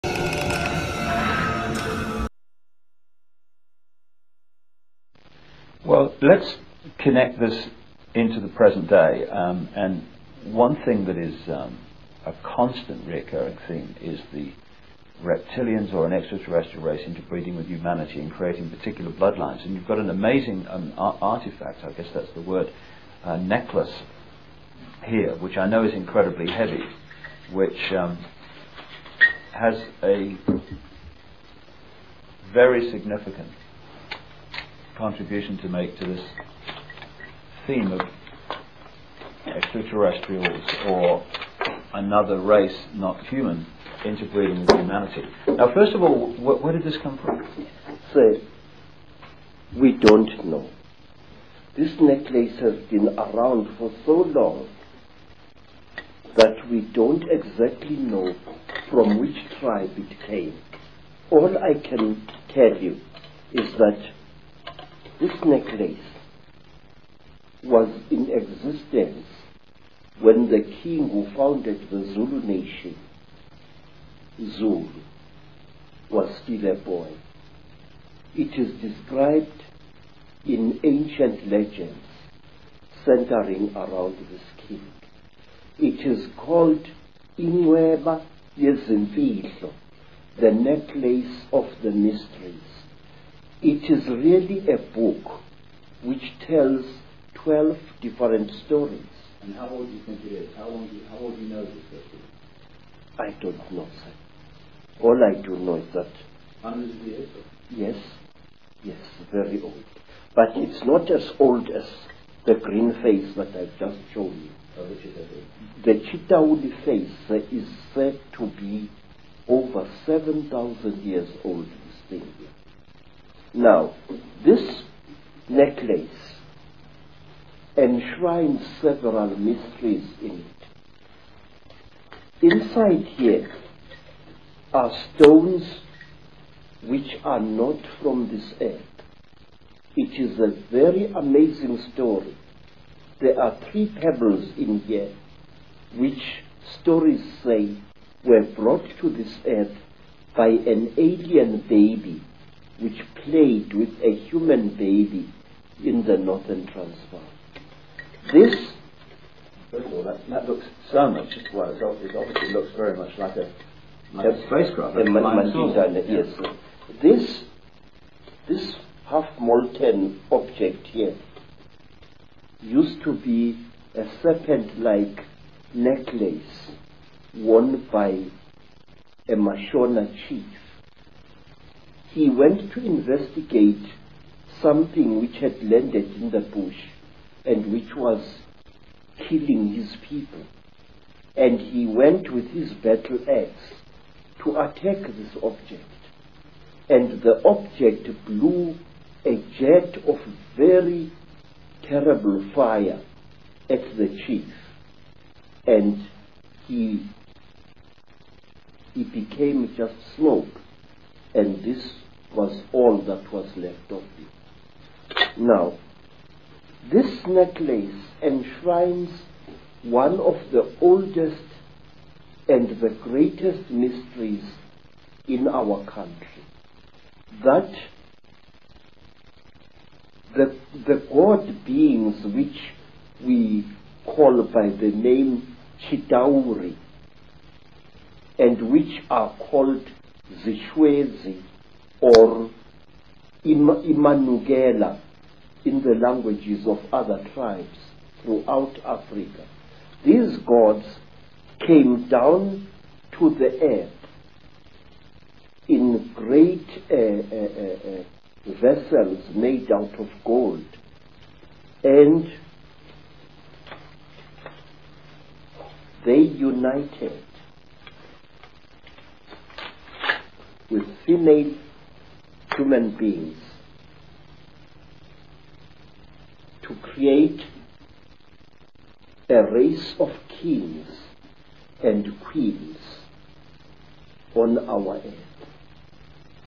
Necklace of the mysteries
The Necklace of the Mysteries in the possession of Credo Mutwa that appears in accounts going back 500 years
and Credo believes is at least 1,000 years old.
The necklace features a non-human figure with a ·come-and-get-me· penis that fits into the earth woman.
This symbolizes the interbreeding of the Chitauri/Anunnaki that created the hybrid bloodline
Another symbol hanging from the necklace is a 'flying saucer' with three non terrestrial rocks inside. that the accounts say were used by the Chitauri/Anunnaki